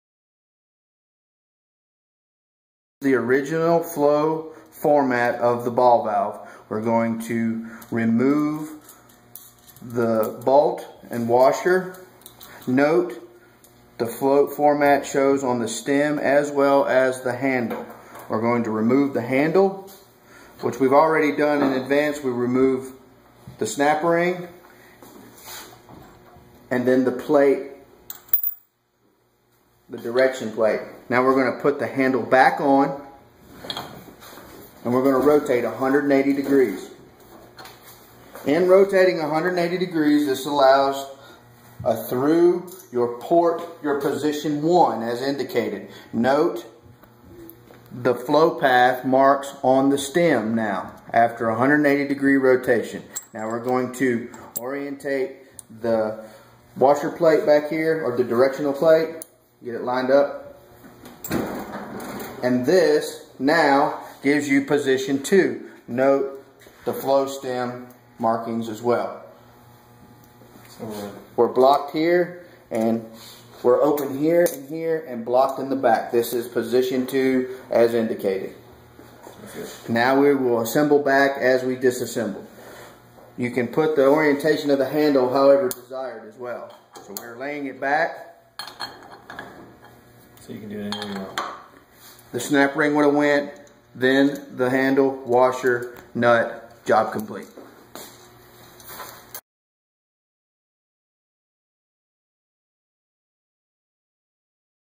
How to Change Orientation of T-Port Ball Valve
This video demonstrates how to change the orientation of a T-Port ball valve.